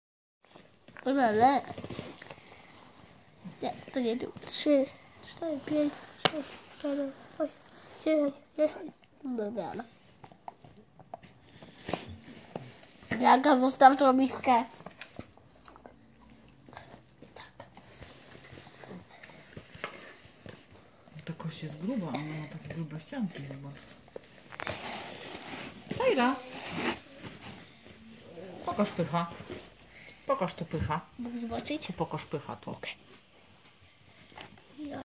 PORANEK - Hodowla Akit Dworska Ostoja
Taka sytuacja o poranku :) Tejin, mała Taira i kość, która jak widać nie jest kością niezgody. Wspólne zabawy dla akit są przyjemną rozrywką, ale też służą budowaniu dobrych relacji z członkami stada.